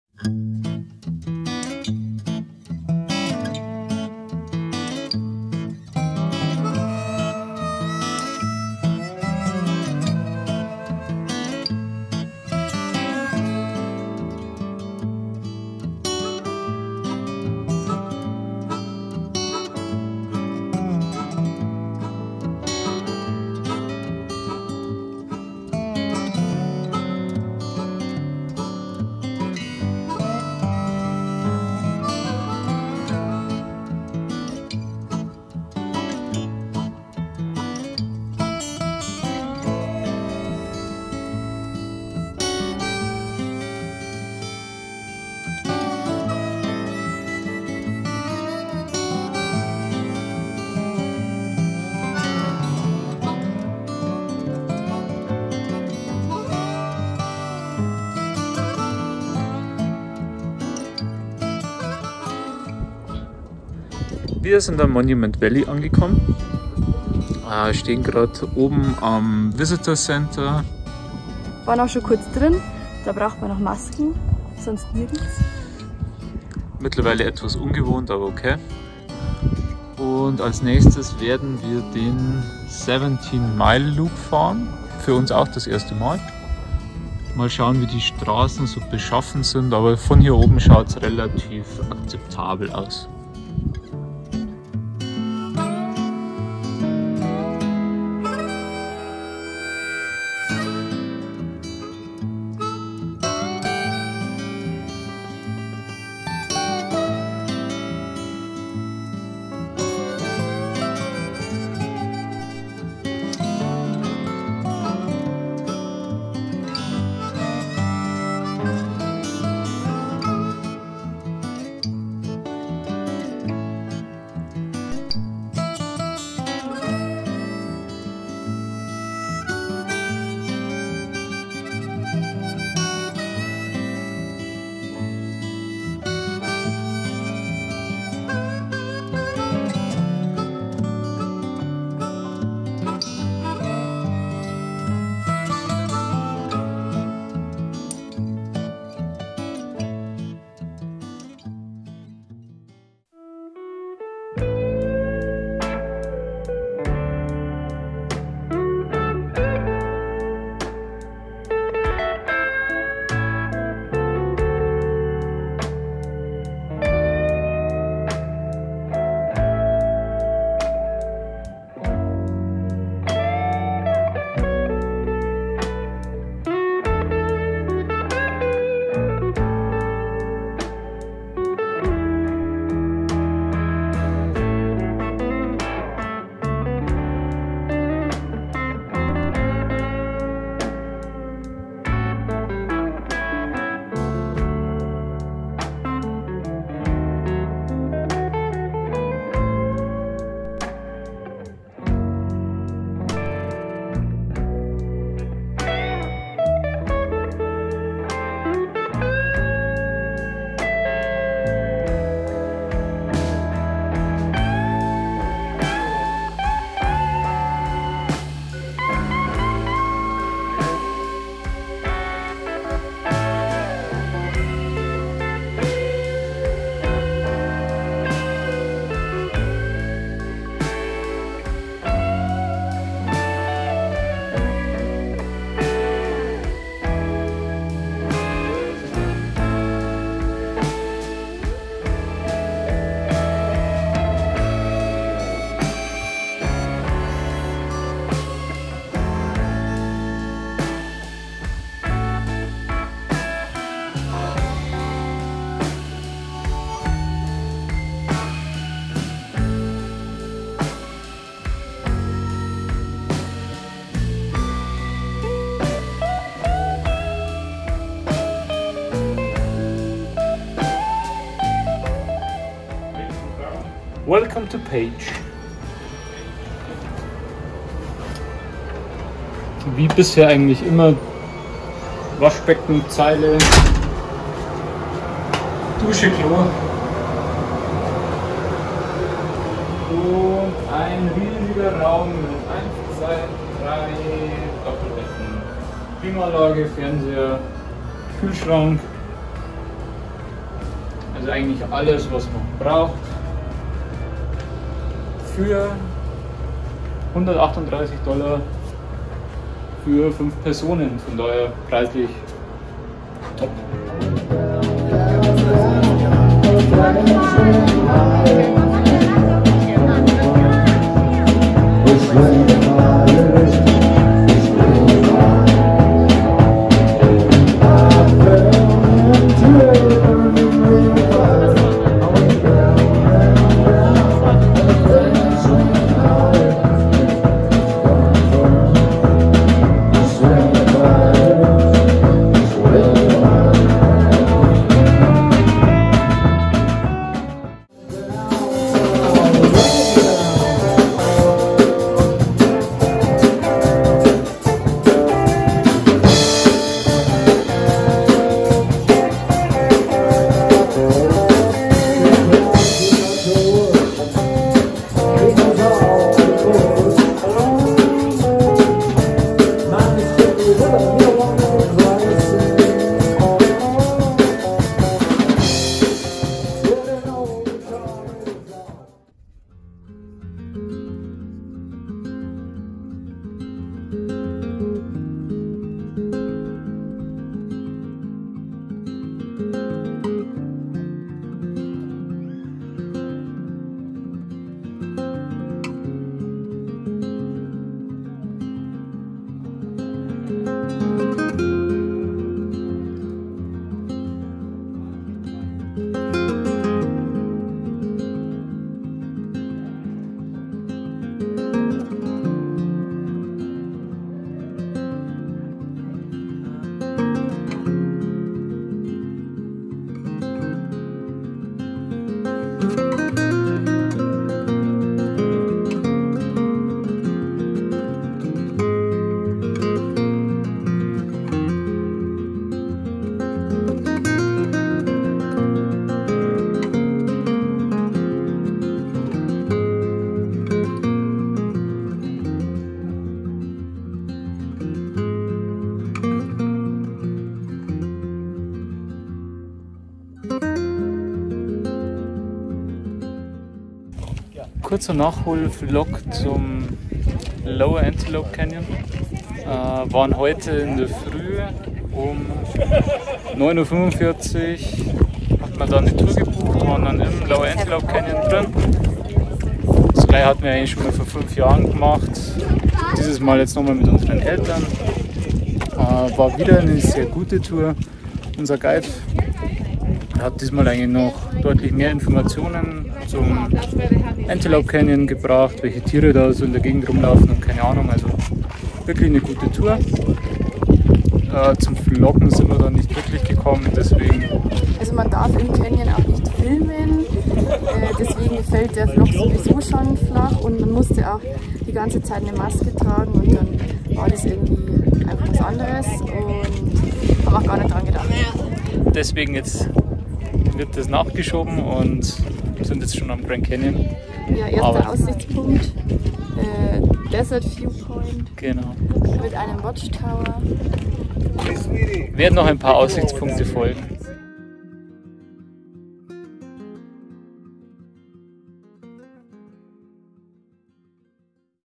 Monument Valley und Antelope Canyon • USA Westküste Vlog 010
🎬 IN DIESEM VIDEO:
Die nächsten Stopps auf unserem Roadtrip führten uns ins Gebiet der Navajos. Das berühmte Monument Valley erkundeten wir diesmal auch auf dem 17 Mile Scenic Drive und fuhren direkt ins Valley. Anschließend war es mal wieder Zeit für den Lower Antelope Canyon, einen wahnsinnig beeindruckenden Slot-Canyon.

Wir freuen uns, wenn euch das Video gefallen hat :)
Nicht vergessen auf die Glocke zu klicken, damit ihr rechtzeitig benachrichtigt werdet ;)